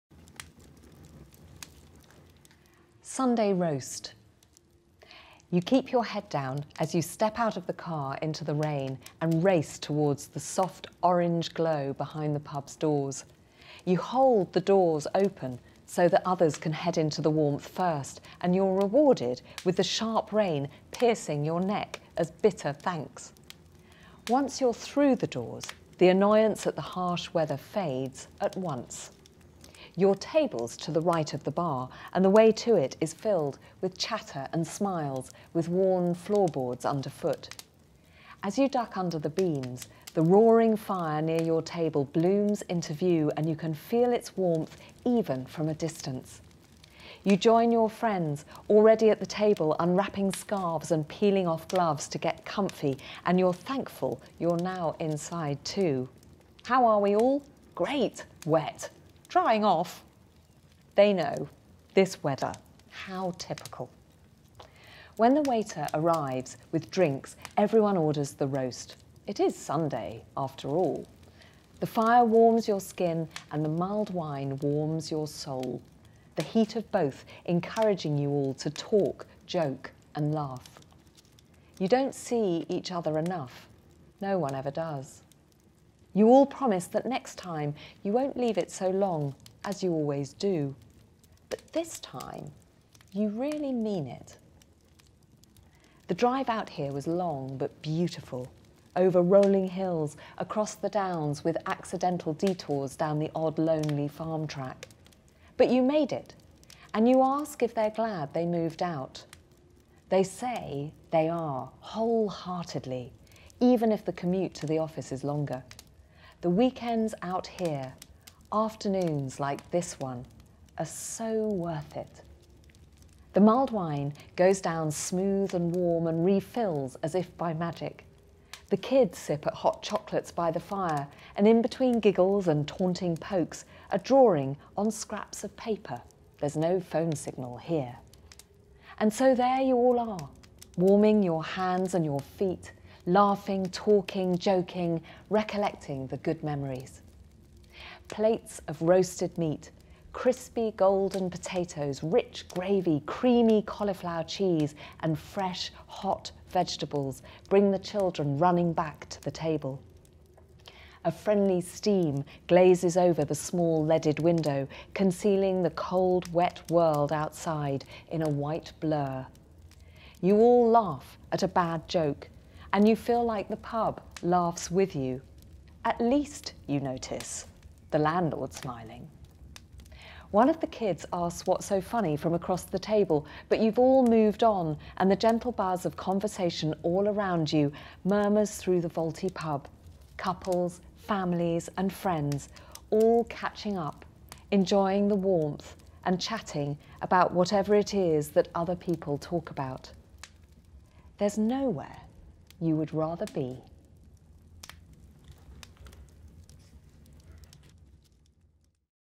Fireside Storytelling with Kate Humble - Sunday Roast
Join Kate Humble as she reads "Sunday Roast" - the fourth of our cosy Fireside Stories. Each Sunday evening, Kate narrates a new heartwarming story.

For pure cosiness throughout your home, think Contura.